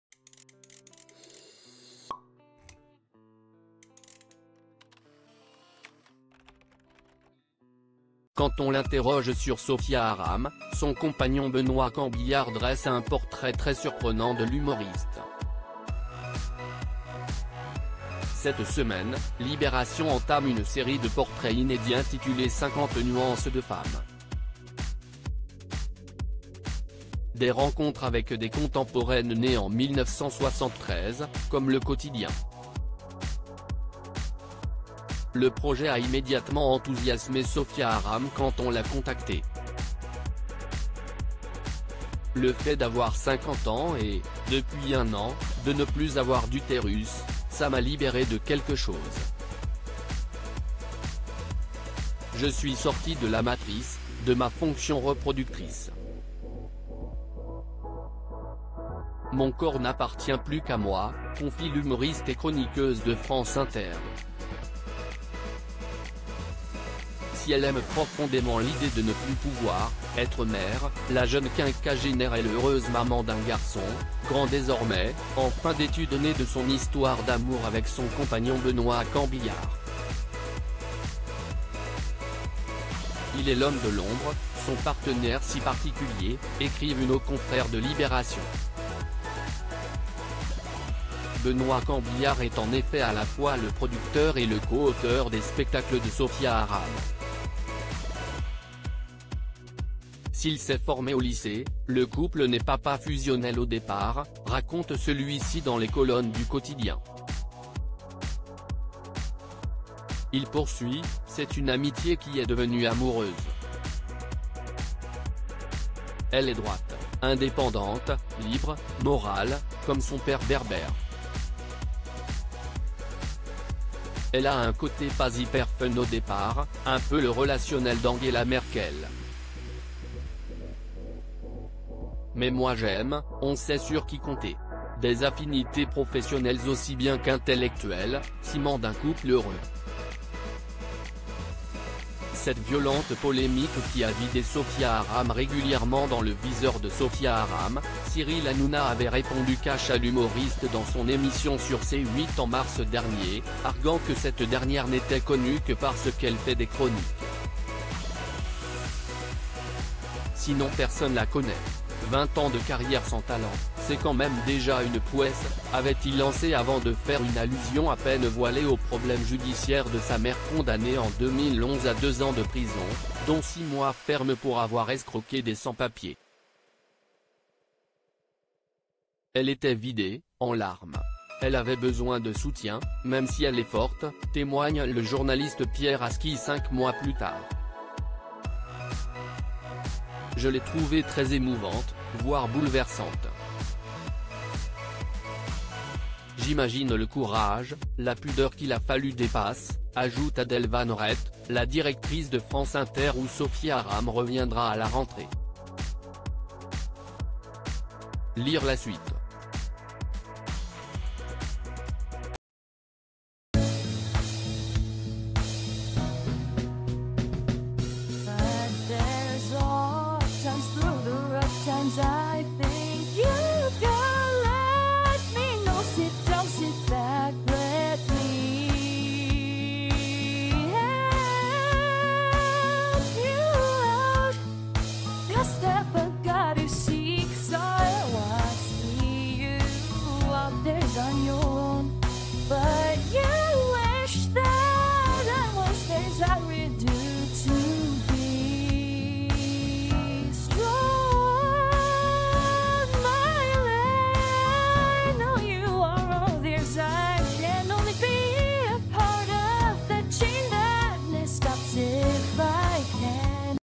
Sophia Aram « Pas hyper fun… », les confidences très inattendues de son compagnon Benoît Cambillar
Sophia Aram : « Pas hyper fun… », les confidences très inattendues de son compagnon Benoît Cambillard